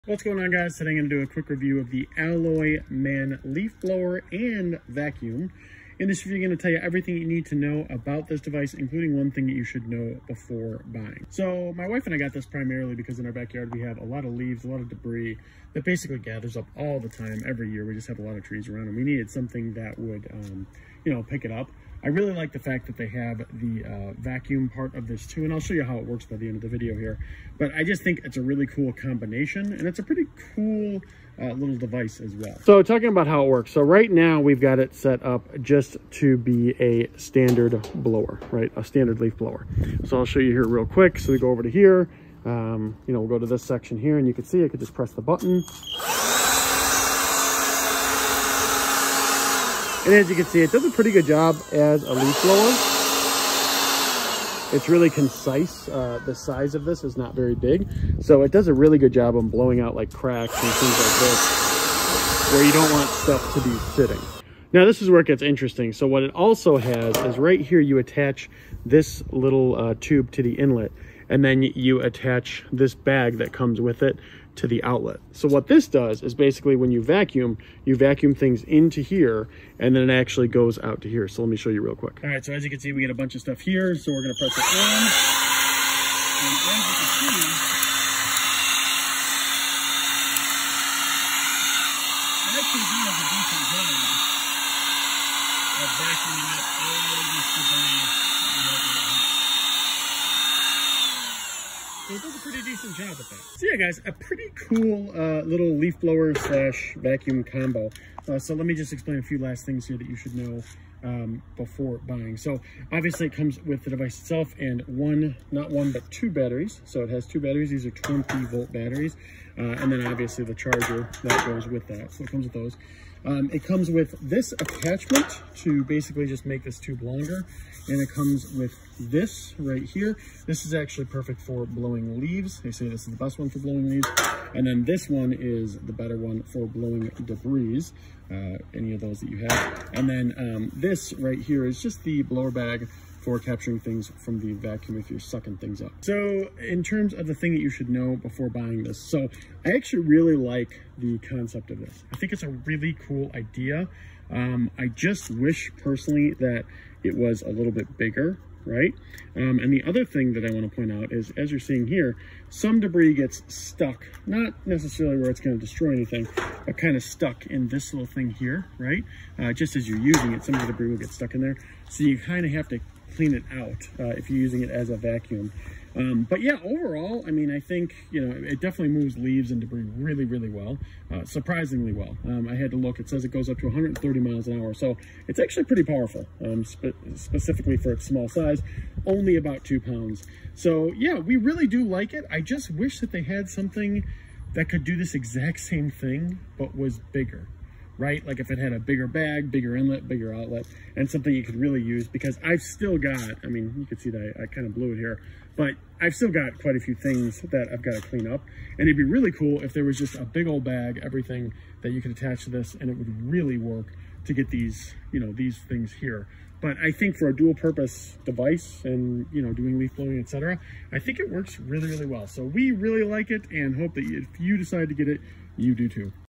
ALLOYMAN Leaf Blower Review - Is It Worth It?
Check out the ALLOYMAN Leaf Blower! This 20V cordless gadget isn't just a leaf blower but also a vacuum, perfect for yard clean-ups and even light snow blowing. Its powerful punch reaches 130 MPH/150 CFM, and it comes with two 4.0Ah batteries, which, by the way, are compatible with Makita. The adjustable nozzle suits all users, whether you're sweeping leaves or dislodging stubborn debris. It's a lightweight, 2-pound wonder with low noise levels, so no ear protection needed. Plus, you get a charger, storage bag, and a full year's warranty.